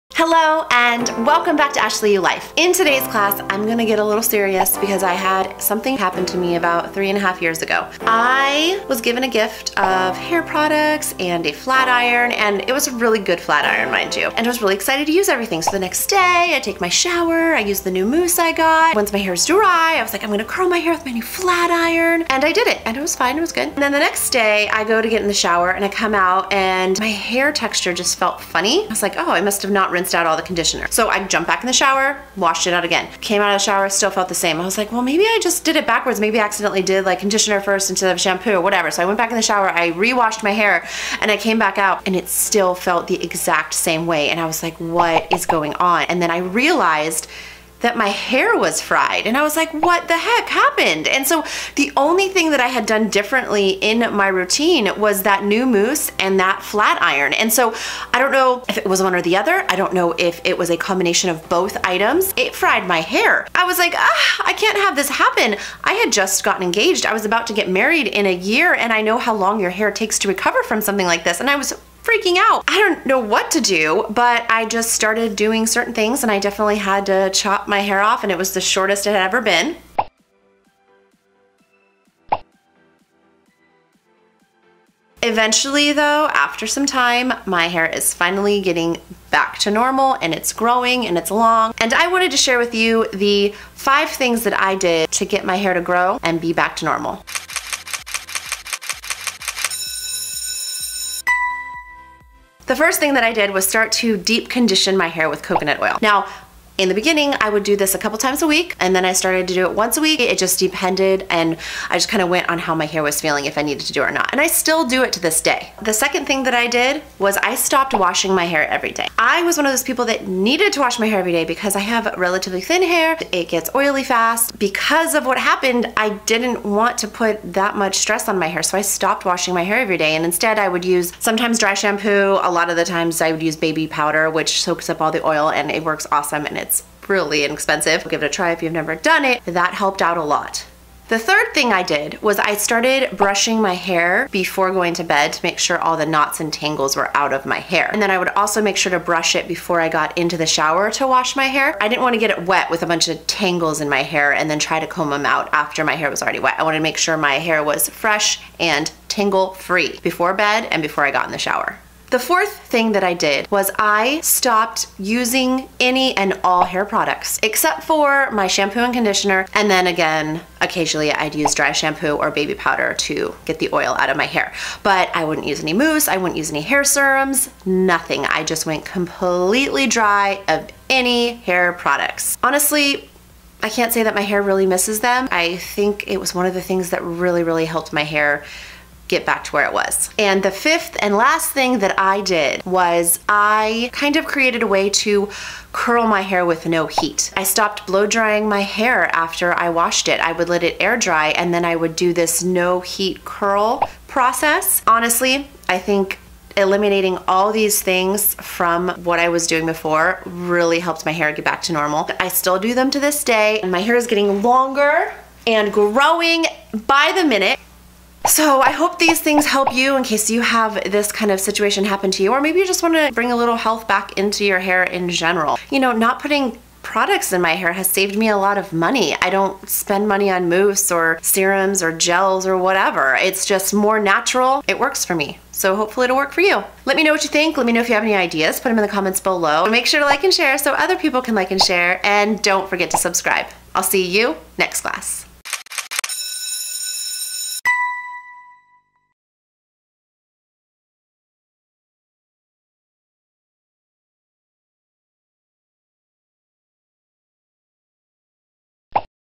5 Easy Steps To Healthier Hair!?!
Did you fry your hair? Maybe your hair could just use a little love to make it feel more healthy! Either way, I have the 5 easy steps to lead you to healthy hair!

See U next Class!

In this class we get serious and talk about how to heal damaged hair. I fried my hair with a flat iron or a new mousse or both I'm not sure. All I know was the damage was done and I had to fix it. So I found 5 simple things I could do that over time returned my damaged hair to a healthy hair.  Now I'm back to doing all the hair styles I love not to mention my hair is growing long and strong again!